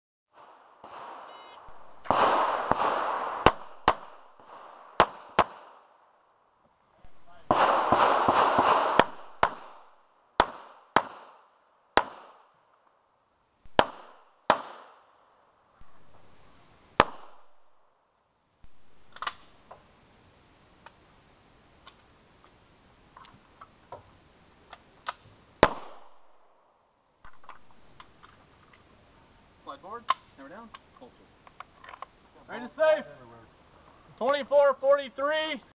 STEVE O. SHOOTING ACTION PISTOL AT K.R.G.C.
BRANDON E. ON THE TIMER.